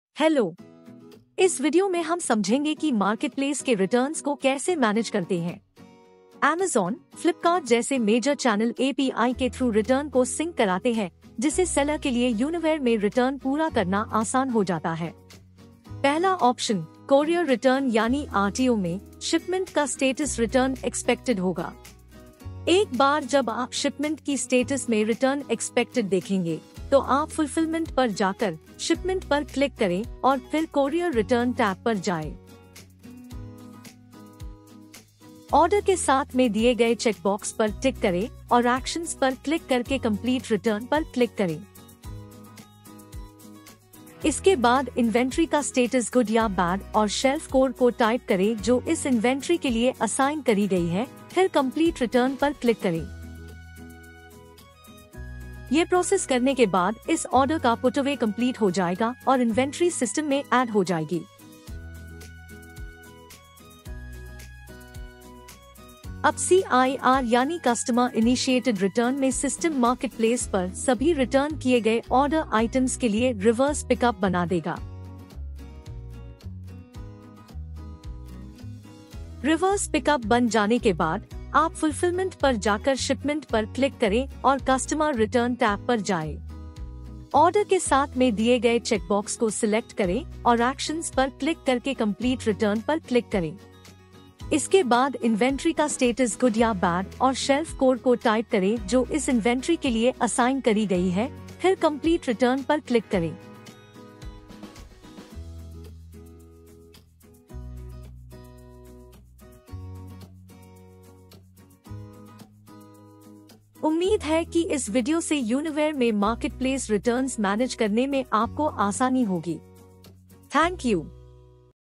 Standard process to receive B2C RTO and B2C Customer Returns – Hindi Version
This Video will help the sellers to Manage the Returns which are Synced directly from the Marketplaces, Carts and custom websites and allows the Sellers to manage the Inventory using Putaway